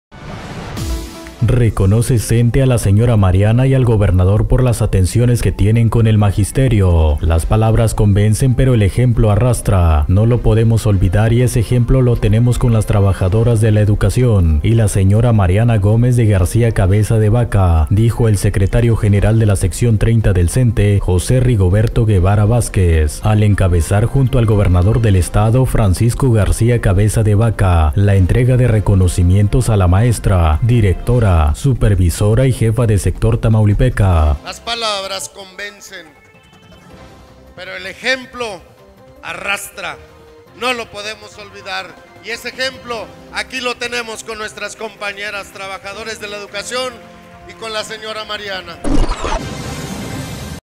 Reconoce SNTE a la señora Mariana y al Gobernador
🚨 HOY EN LA RED: Reconoce SNTE a la señora Mariana y al Gobernador por las atenciones que tienen con el magisterio.